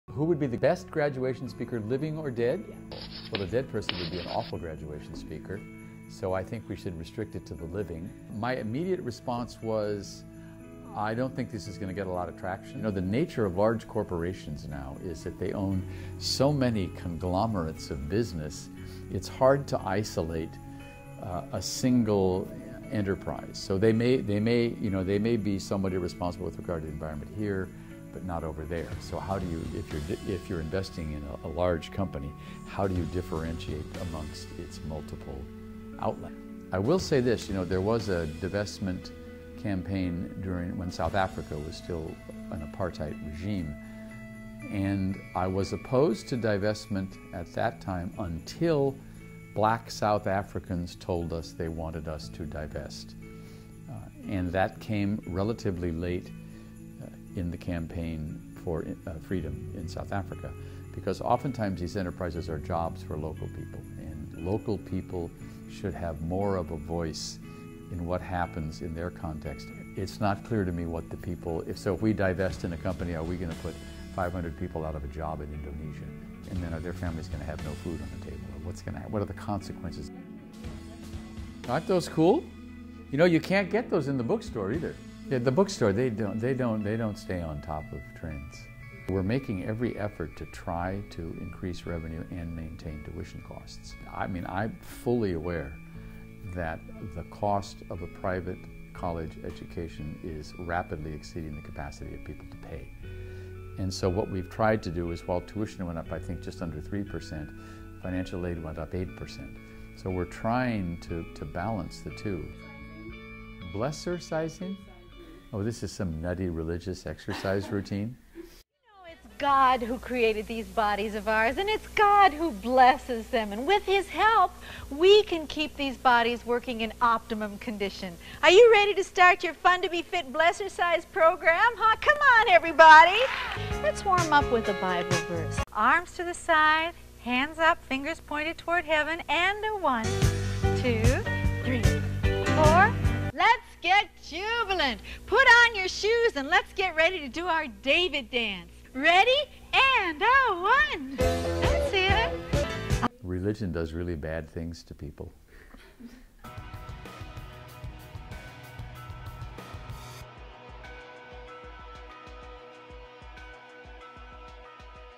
Talks: Ask the President (Episode 4)
USFtv interviewed Father Privett using YOUR questions! Episode 4 includes talks about divestment, rising tuition costs at USF for the '13 - '14 school year and the '80s trend of "Blessercizing"